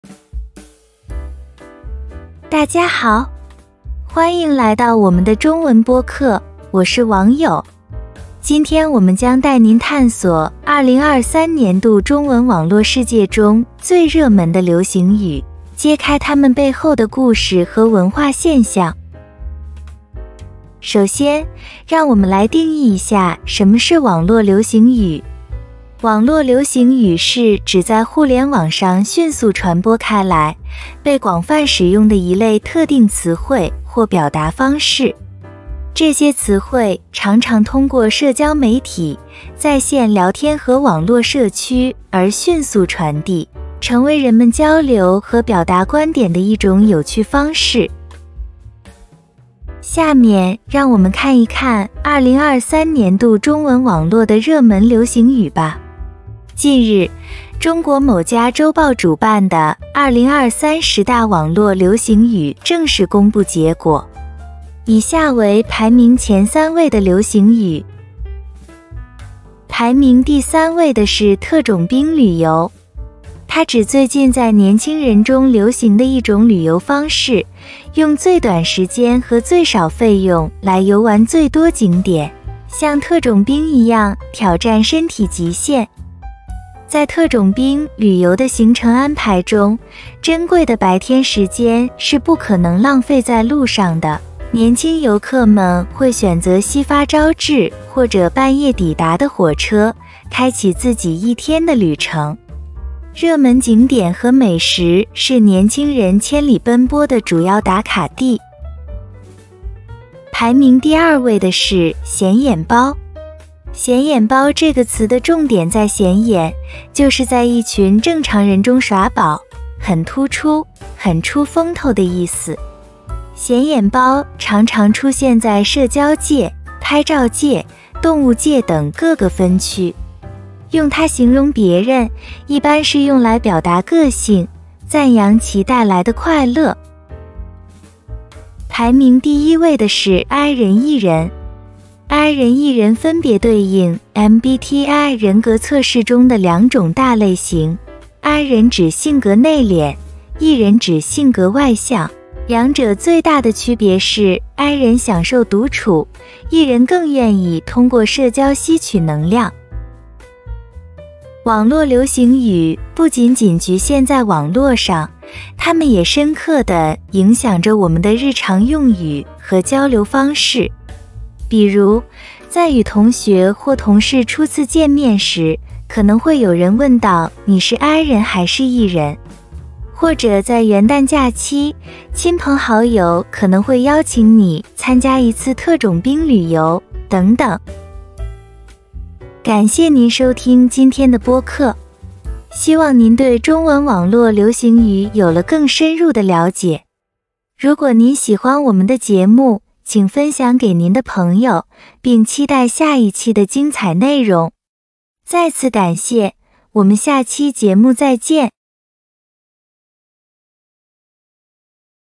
[Advanced Chinese]Wangyou Chinese Podcast 12(王友中文播客第十二期)：Internet Slang(网络流行语）
Hello everyone, I am Wang You, and I am very happy to meet you.
The theme of this podcast is internet slang, I hope you like it.
大家好，我是王友，很高兴与大家见面。
本期播客的主题是网络流行语，希望大家能够喜欢。

📌 You can listen to my podcast on Spotify too!